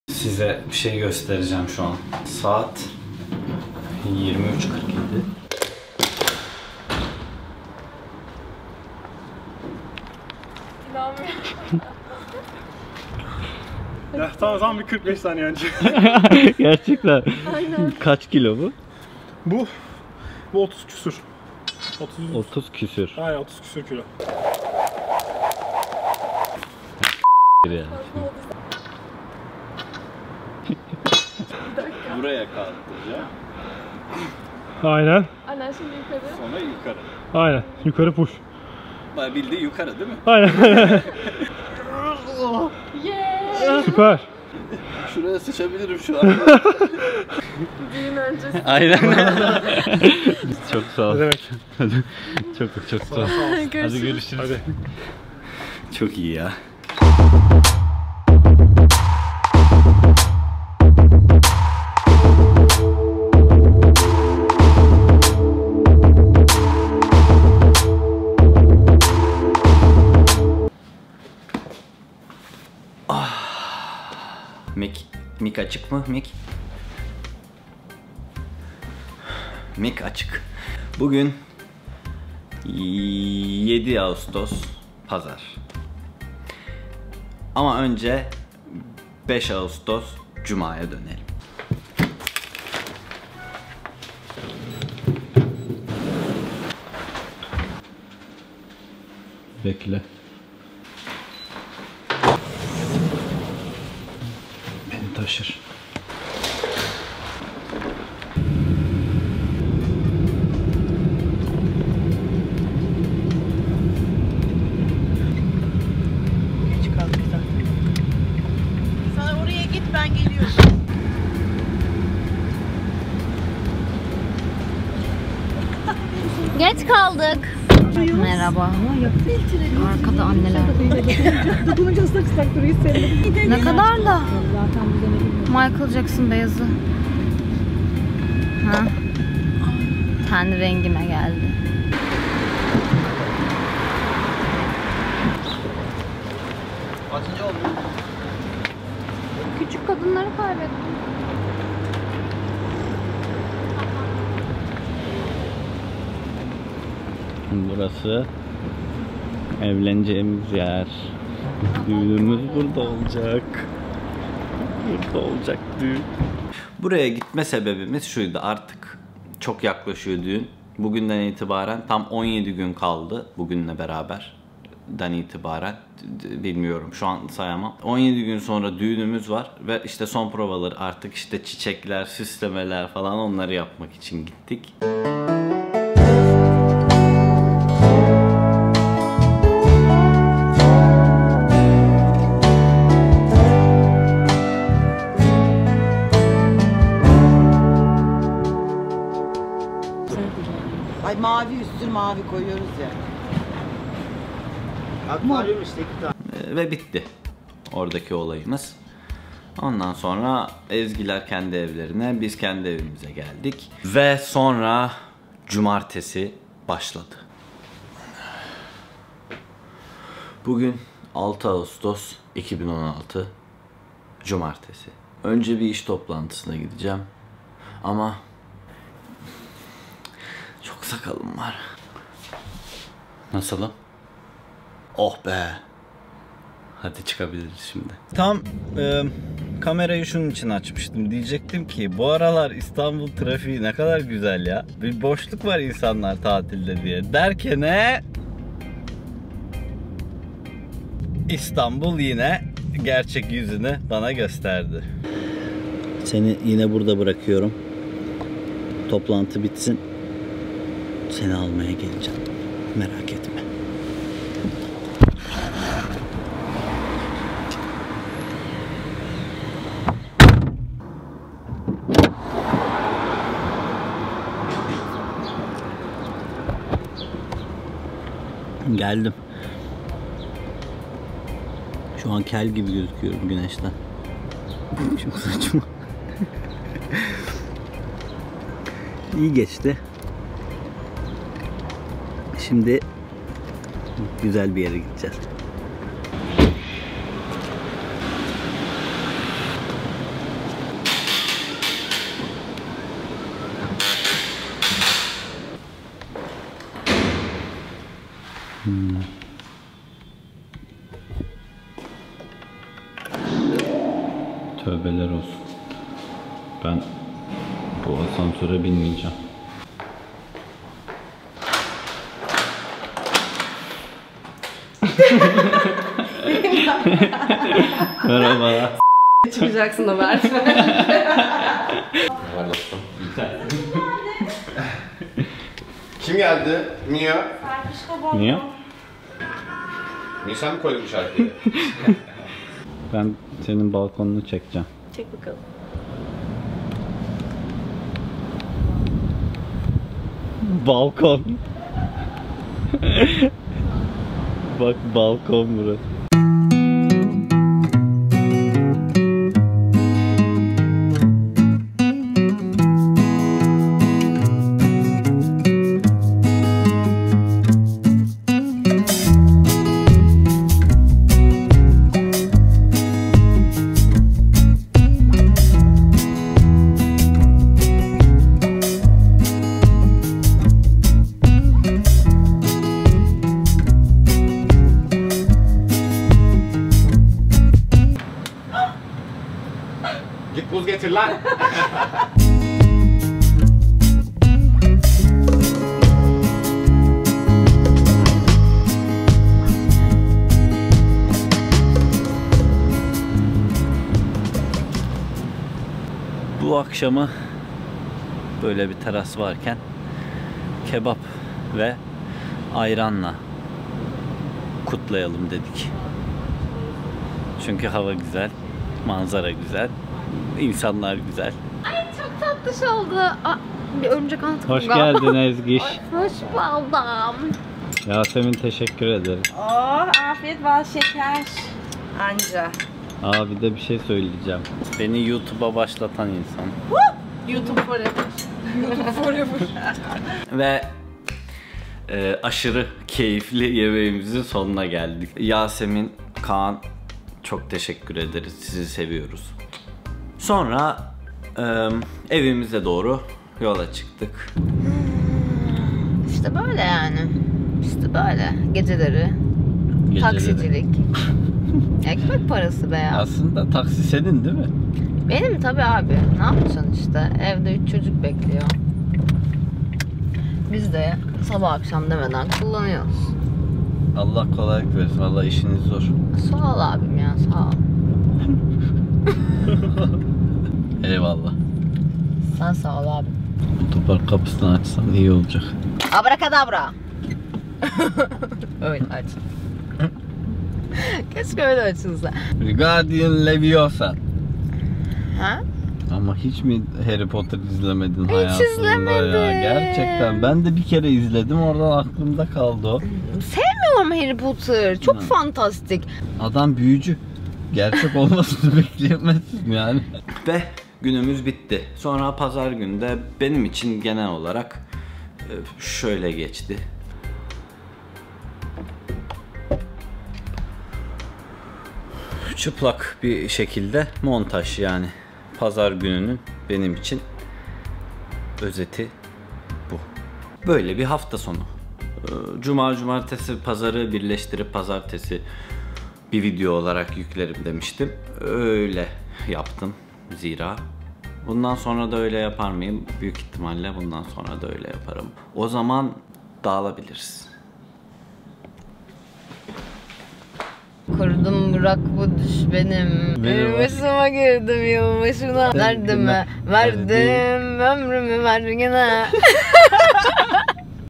Taksi Senin Mi? | VLOG #10
Cuma, Cumartesi ve Pazar'ı birleştirdim. Düğüne 17 gün kala son hazırlıklarımızı tamamlamak üzereyiz. Artık resmen heyecanlıyız.